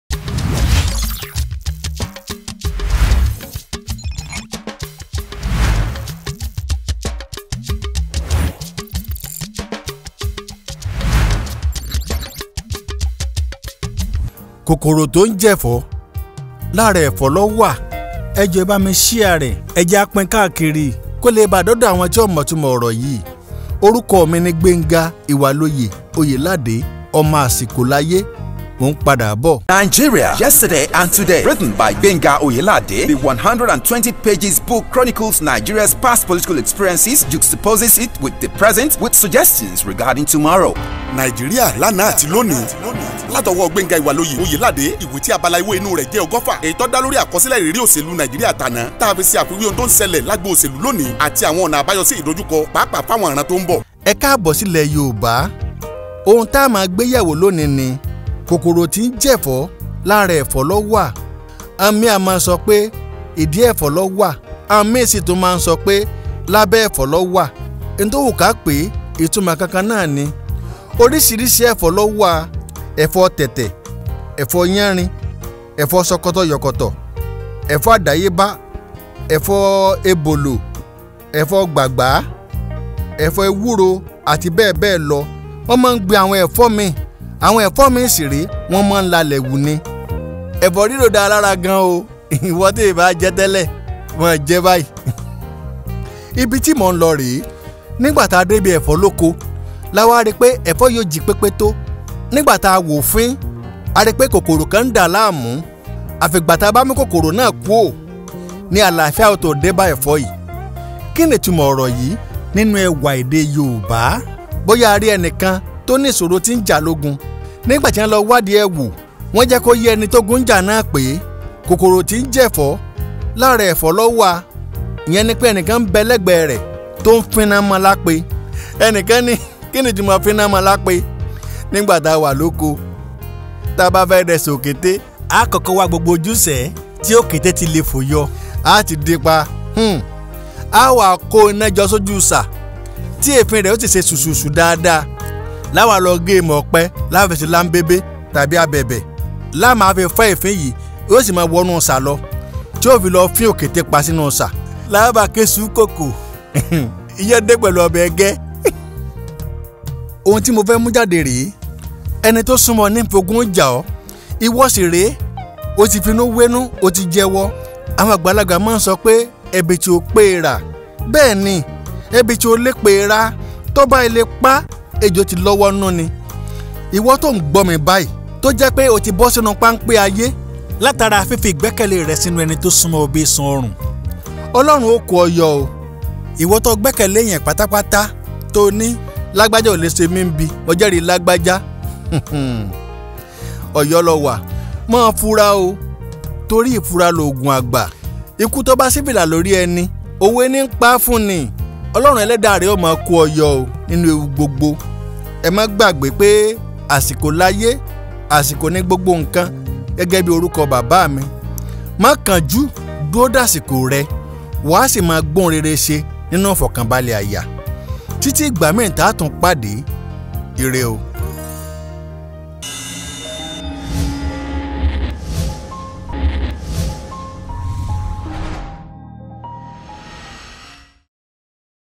KÒKÒRÒ TÍ ŃJẸ̀FỌ́ LÁRA Ẹ̀FỌ́ LÓ WÀ (The insect that is eating a vegetable is in its body)
Meaning that sometimes people very close to you may plan evil against you and you can never imagine they can do such. Please be careful and watchful. Don't trust people 100%, because people can be funny at times. God is your strength